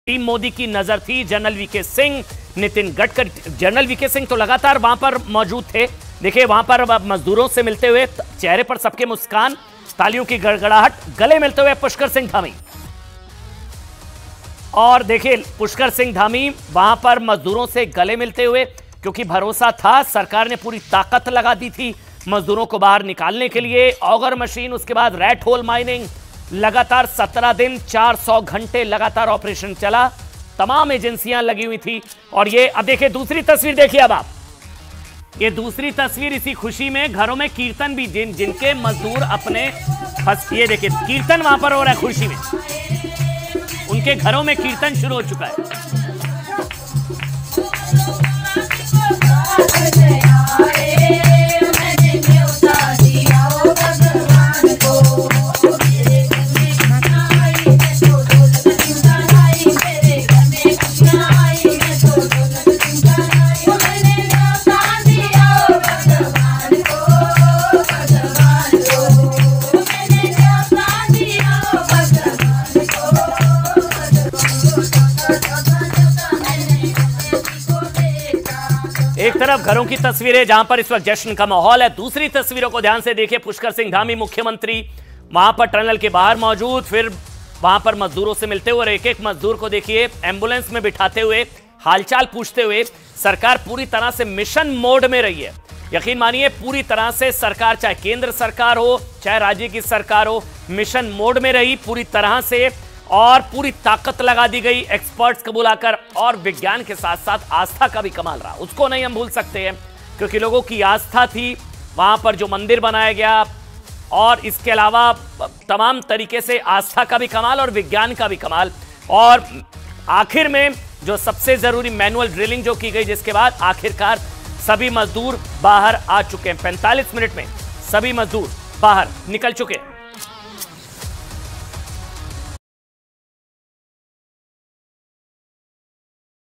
Uttarkashi Tunnel Rescue | 17 दिन बाद निकले मजदूर, मायूस चहरों पर आई मुस्कान | News18
Uttarkashi Tunnel Rescue | फंसे मजदूरों के निकलने के खुशी में परिजनों के घर भजन कीर्तन | News18